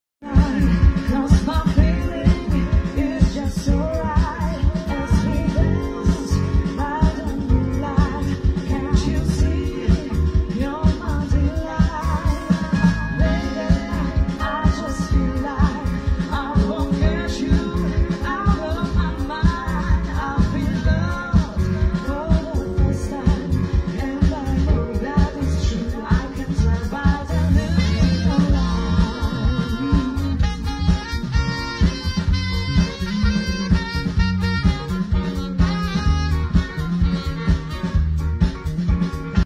Piknik rodzinny, na scenie Julia Węgrowicz i zespół Decover.
Na scenie Julia Węgrowicz i zespół Decover. Warszawa wieczorna wypełnia Park Marszałka Edwarda Rydza.